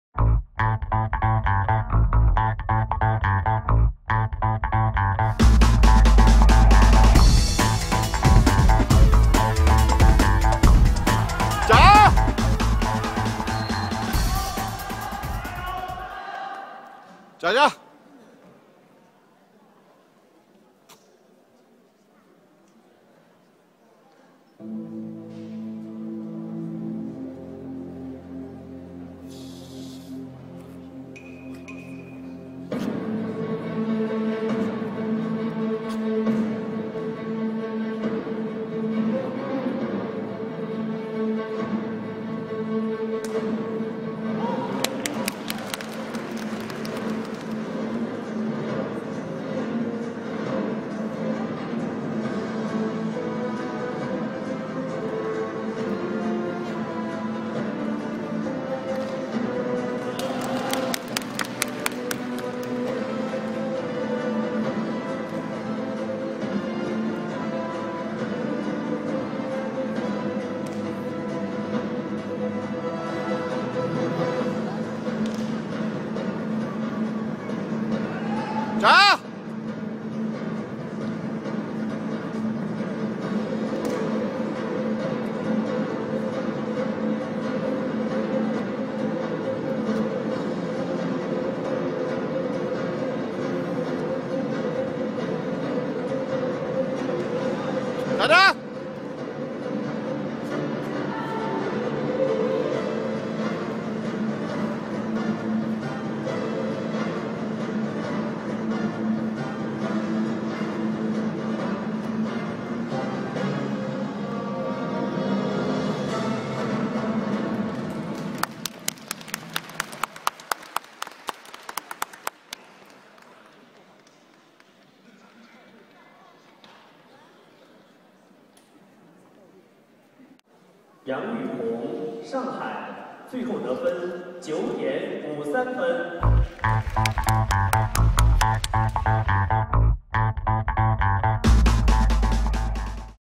1 Yang Yu Hong 杨屿泓 (Shanghai 上海) - Men's Changquan - 9.53 (5)
-- An athlete competing at the "Top 8" Wushu competition in Lanzhou, Gansu, China on October 27-28, 2012.  This competition brought some of the best wushu athletes from China together to compete in over 20 different competition events.